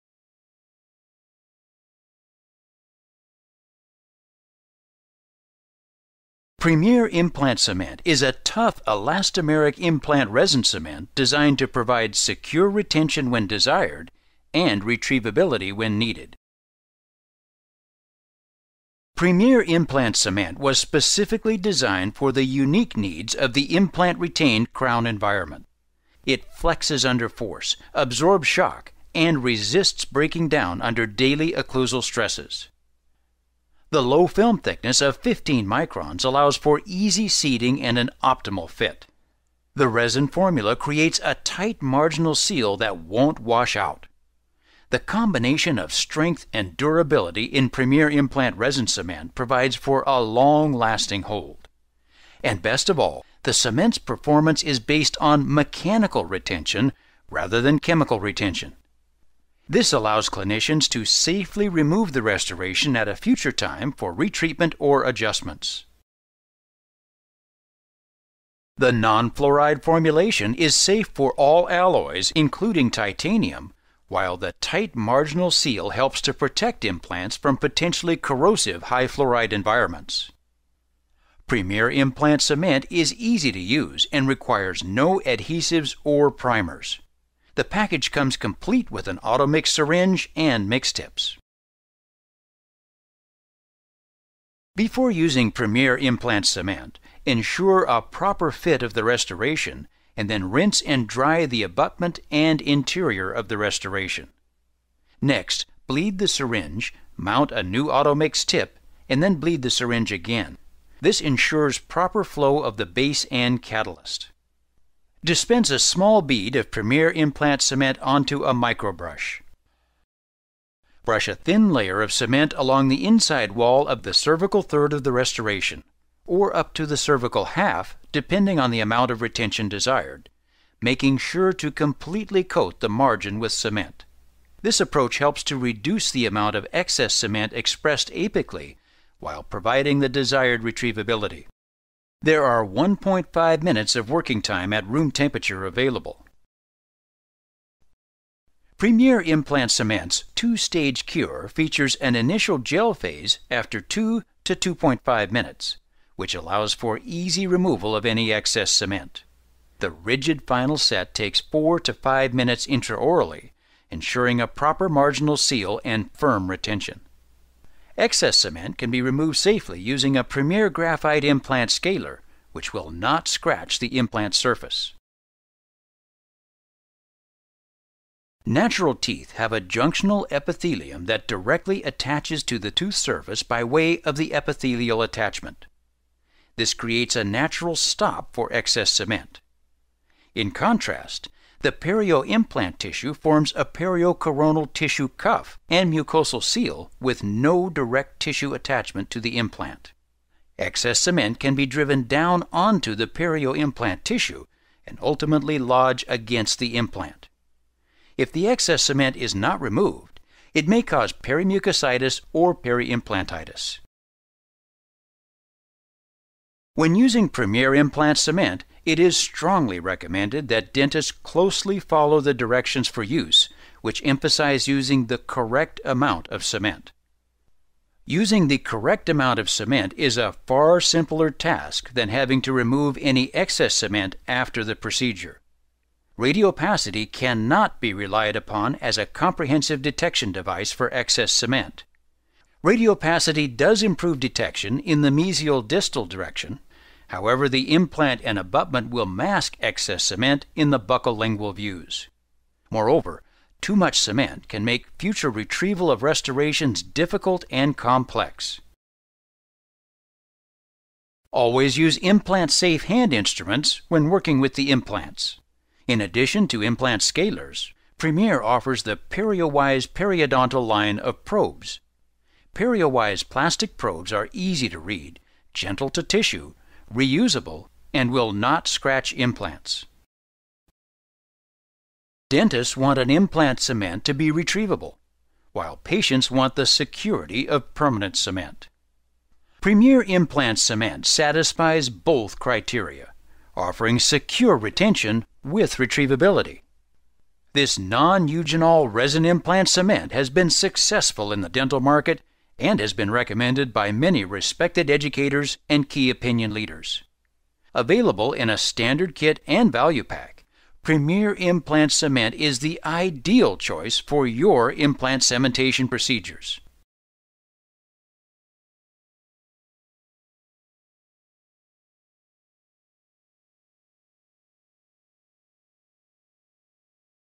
Premier Implant Cement
Non-Eugenol, Temporary Resin cement for Implant Retained Crowns.

Standard Pack contains one 5ml Automix Syringe, 10 Mixing Tips and one Mixing Pad.